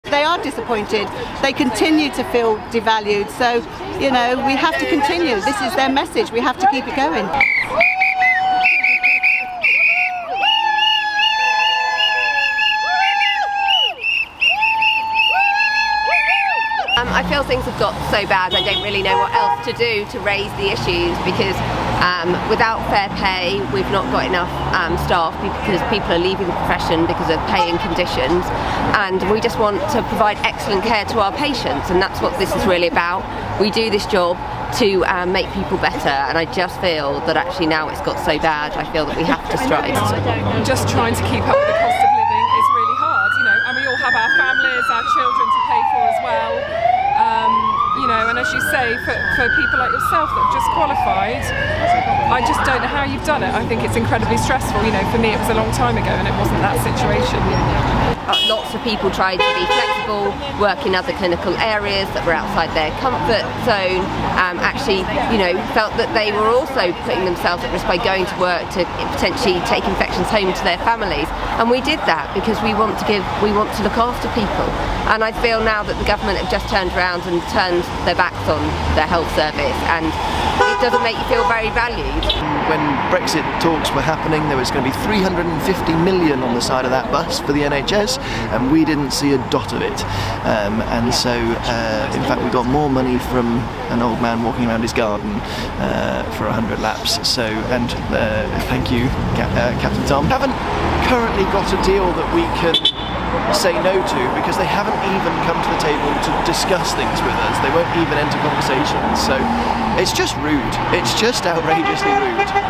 Nurse Strike
As the biggest strike in NHS history took place.
I visited the nurses on strike in Oxfordshire to get their views and their messages to the Government